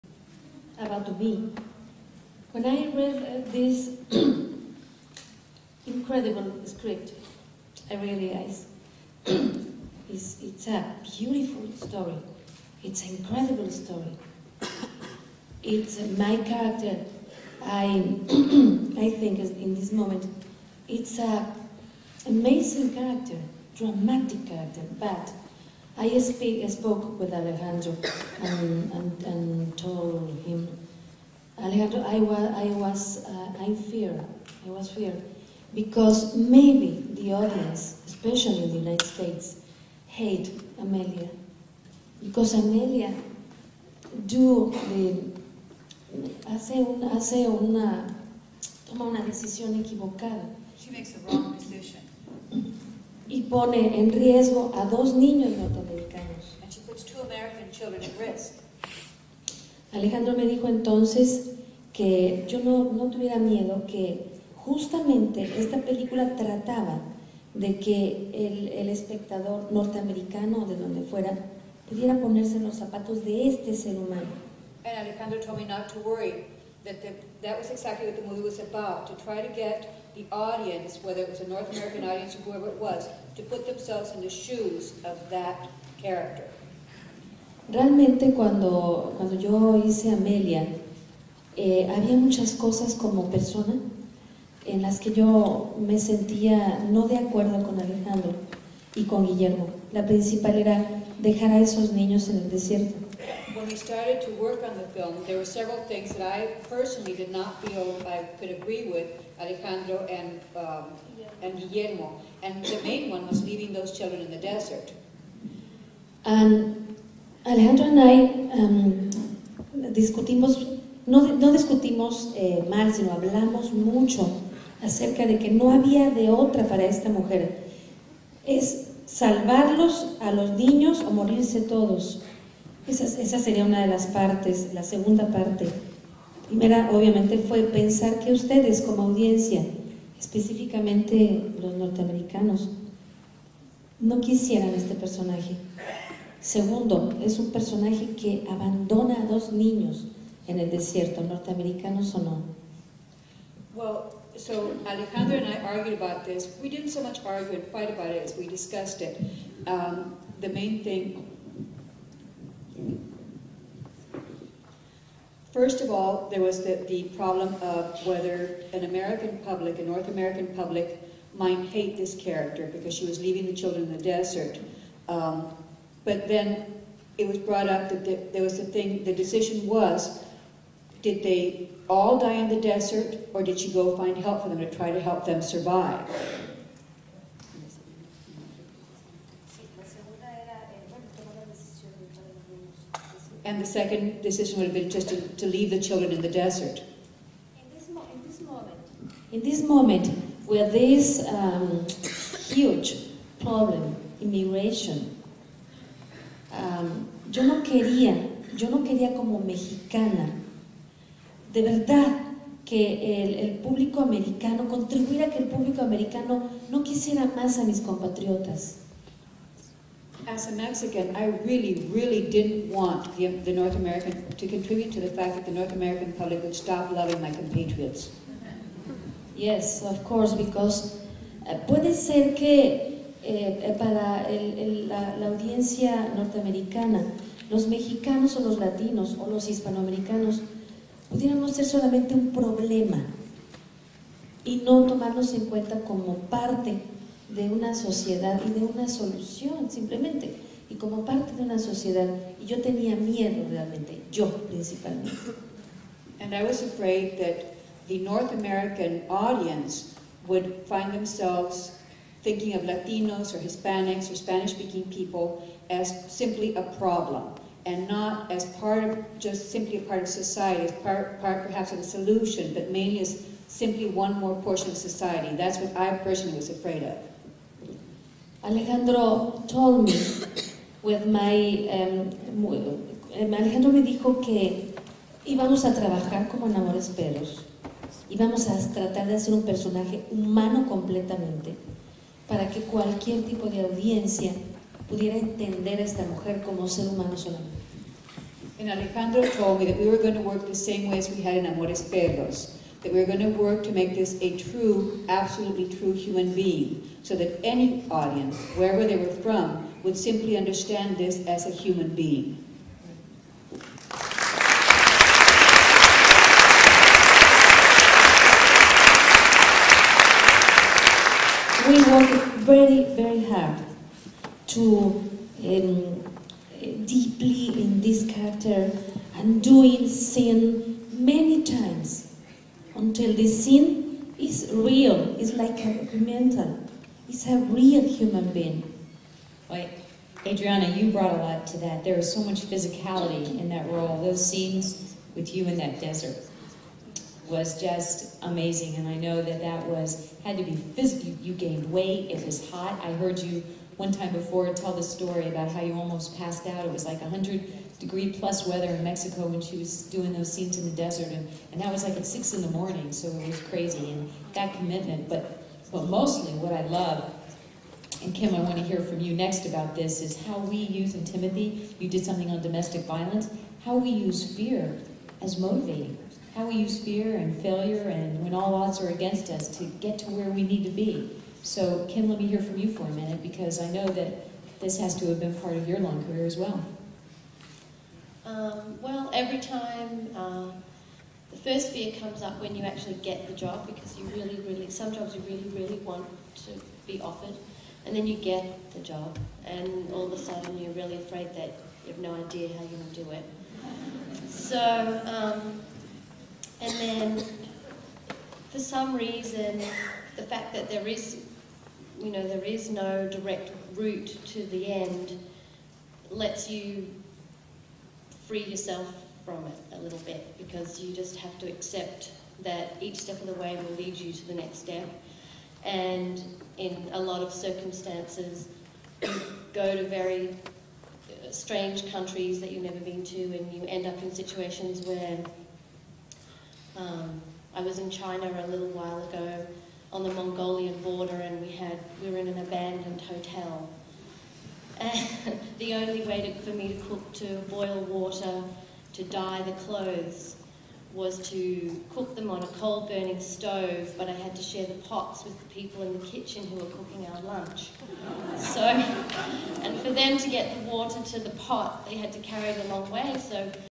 SBIFF Women's Panel Part 9
erated by Madelyn Hammond, Senior Vice-President of Sales, Marketing and Retail for Landmark Theatres, participants so far include Adriana Barraza (actress, Babel), Virginia Katz (editor, Dreamgirls), Cecilia Peck (Director, Shut Up and Sing), Consolata Boyle (costume designer, The Queen), Lesley Chilcott (co-producer, An Inconvenient Truth), Timothy Blake (President of the SAG Film Society), Kym Barrett (costume designer, Eragon/The Children of Huang Shi), Deepa Mehta (director, Water)
Camera and sound by Joseph Matheny and Scott Parent.